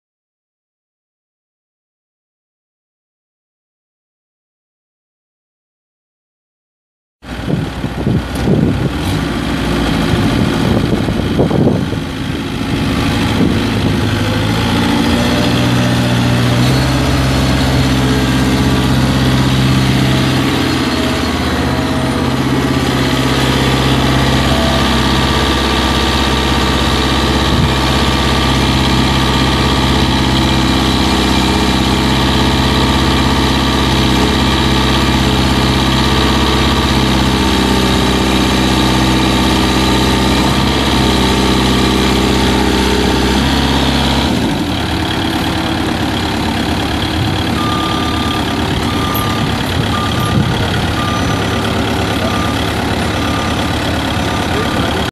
2006 Bobcat Toolcat 5600 utility work machine for sale | no-reserve auction December 28, 2016
2006 Bobcat Toolcat 5600 utility work machine
6,346 hours on meter
1F-1R gears
Four wheel drive
Front and rear wheel steering
Enclosed cab
Auxiliary hydraulics
60"W bucket
59"L x 56"W x 10"H dump bed
Hydraulic dump
2" receiver hitch
27x10.50-15 tires
Rust
Serial A00211135
It's located in Cedar Falls, IA, and we're selling it to the highest bidder regardless of price.